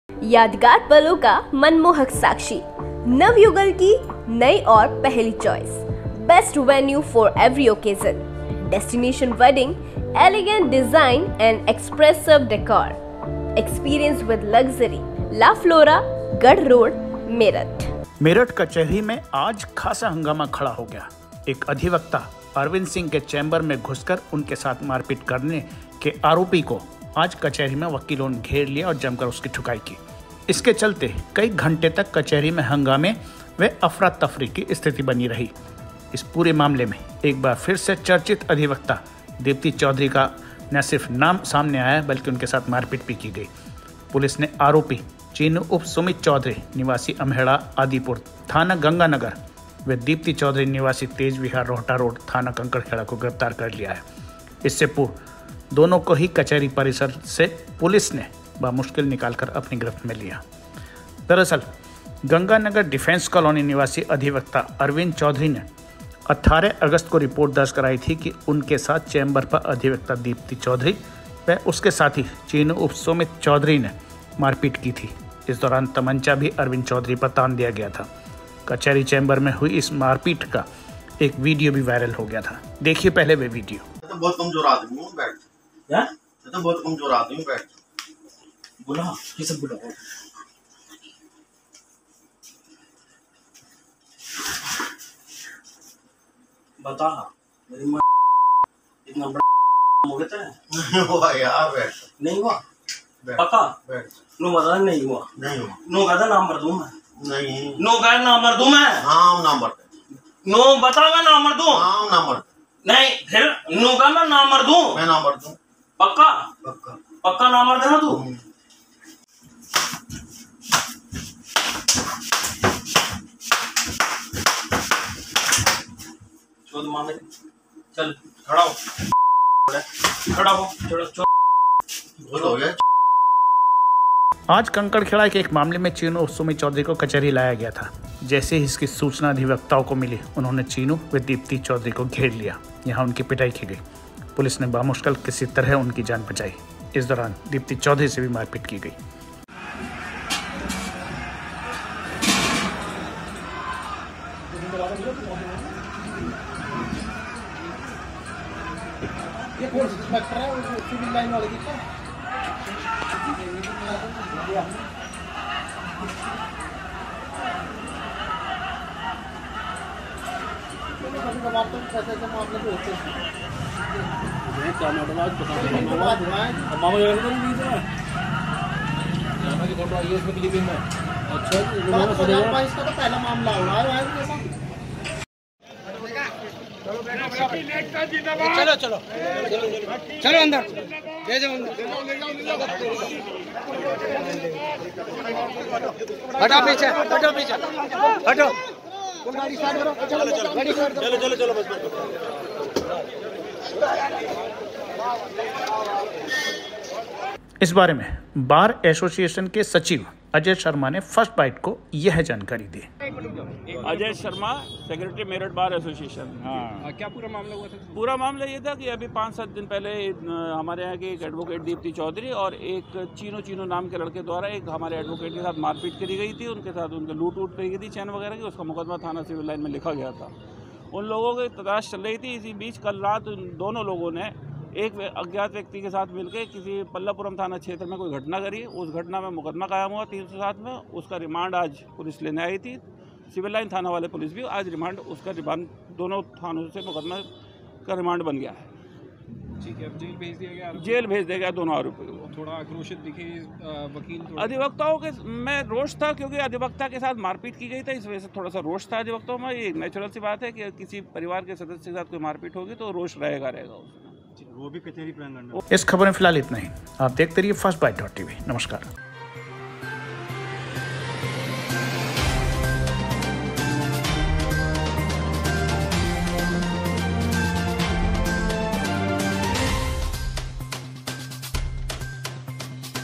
अधिवक्ता की पिटाई करने वाले चीनू की अधिवक्ताओं ने की ठुकाई, कचहरी में हंगामा । Firstbyte.Tv
18 अगस्त को अधिवक्ता अरविंद सिंह की चैंबर में की थी जमकर पिटाई
अरविंद सिंह ने चीनू व दीप्ति चौधरी के खिलाफ दर्ज कराई थी रिपोर्ट
मंगलवार को चीनू किसी अन्य मामले में लाया गया था कोर्ट
अधिवक्ताओं ने घेरकर उसकी की पिटाई
बामुश्किल किसी तरह पुलिस ने उसे वकीलों के चंगुल से बचाया